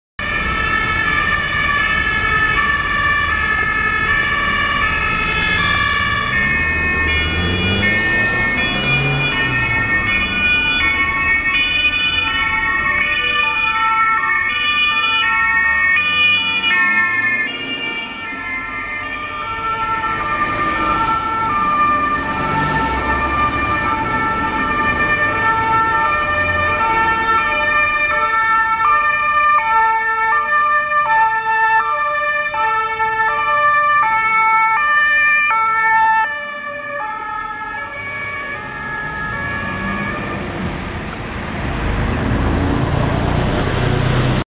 [SEHR SELTEN DOPPEL NEF] NEF Rettungsambulanz Greiz + NEF DRK Zeulenroda [11/2015]
In diesem Video sind 2 Notarzteinsatzfahrzeuge (NEF) von der Rettungsambulanz Greiz und vom Deutschen Roten Kreuz (DRK) Zeulenroda auf Einsatzfahrt gleich hinter einander zusehen. Dies ist sehr selten.